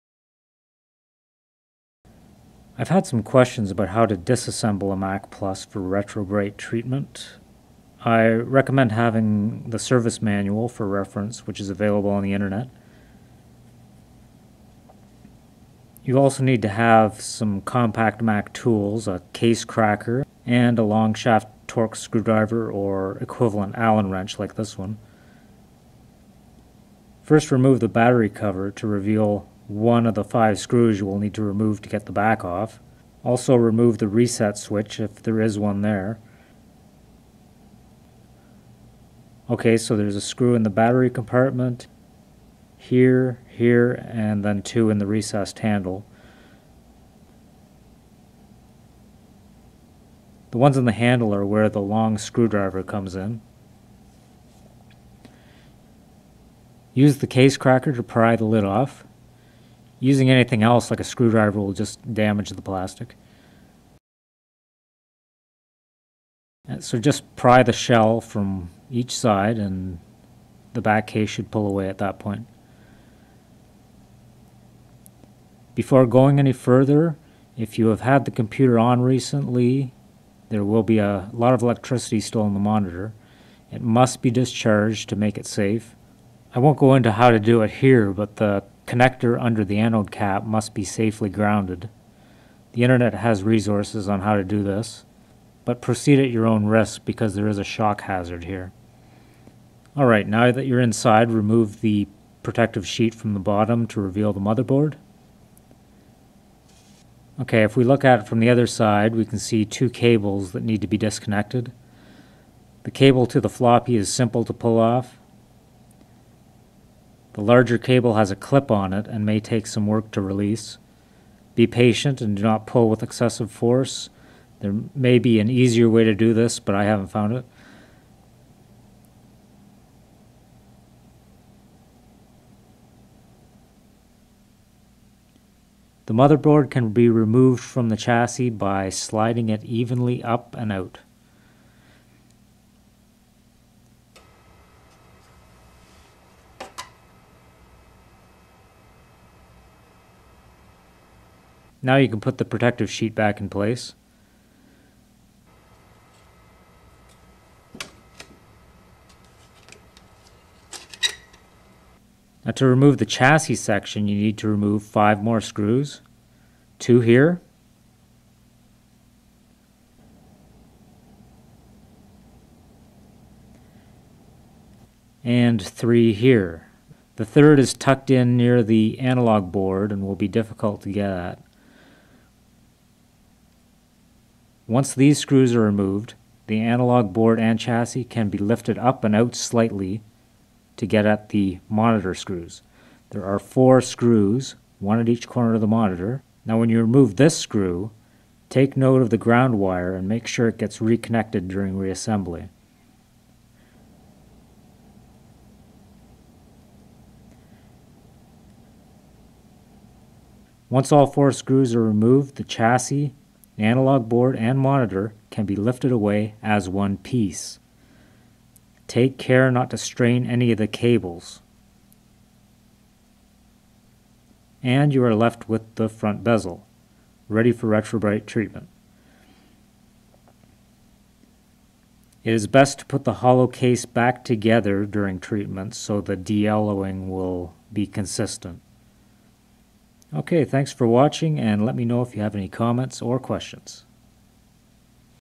Macintosh Plus Disassembly
This is a take-apart instruction for those wanting to disassemble a Mac Plus for the purpose of Retrobrighting (De-yellowing). See my previous video for Retrobright instructions. Disassembly of other compact Macs will be similar.